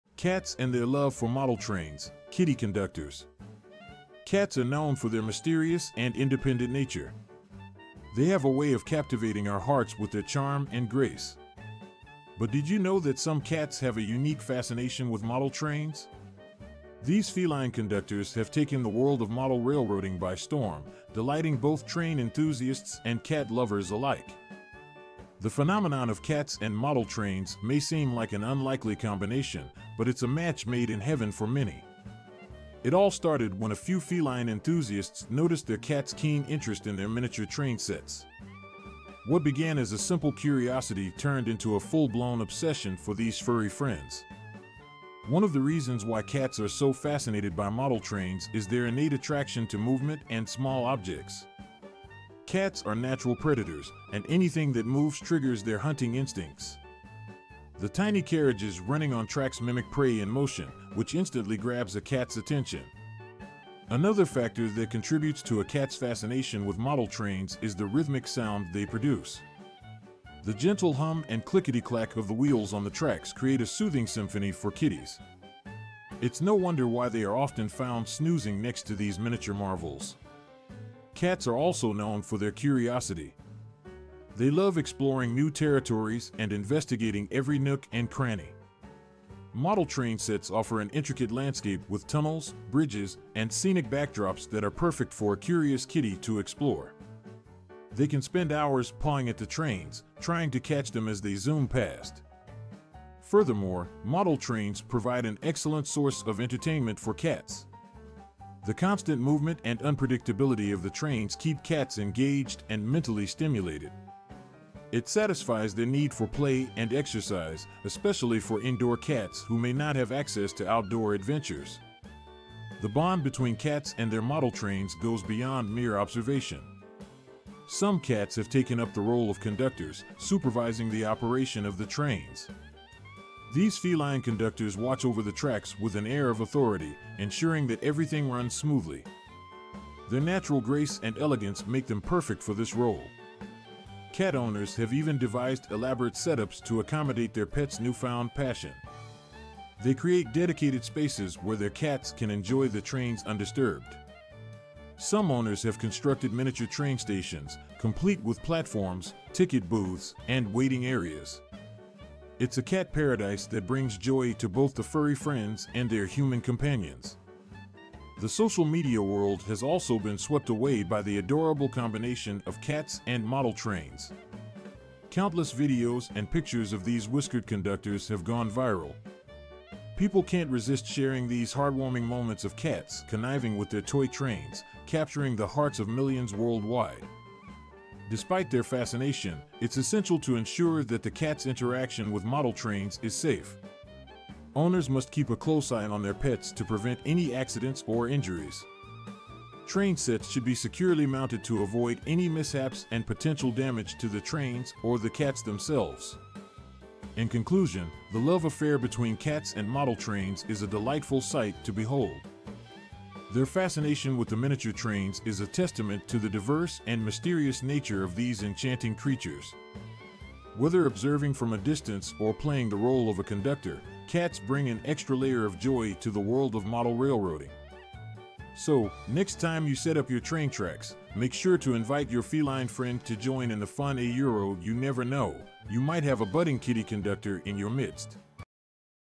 Kitty Conductors: Cats and Their Love for Model Trains
Cats and model trains: a unique combination of fun and entertainment! This strange fascination has delighted both train enthusiasts and cat lovers alike. Cats are known for their independent and mysterious nature, but some cats have taken their love for trains to the next level. Whether it's lying on top of the tracks or playing with miniature cars, cats and model trains make for a perfect combination! Keywords: cats, model trains, train enthusiast, feline conductor, kitty conductor, miniature cars.